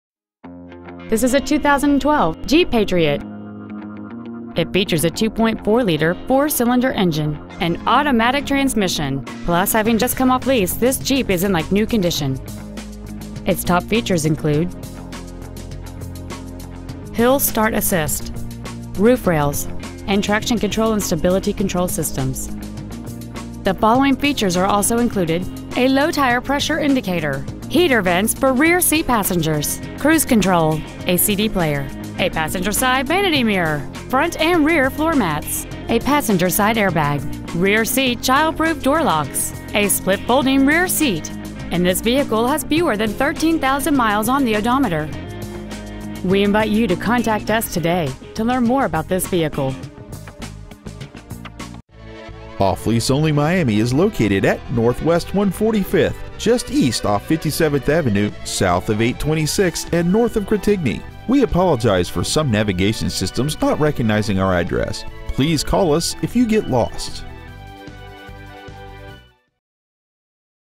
2012 Jeep Patriot Miami Fl
We have been honored to serve the Miami FL area , we promise that your experience at our dealership will exceed your expectations ! Year : 2012 Make : Jeep Model : Patriot Engine : Gas I4 2.4L/144 Trans . : Automatic Exterior : Orange Pearl Miles : 12,606 Interior : Black Stock : 52672 OffLeaseOnly Miami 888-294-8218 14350 NW 56th Ct.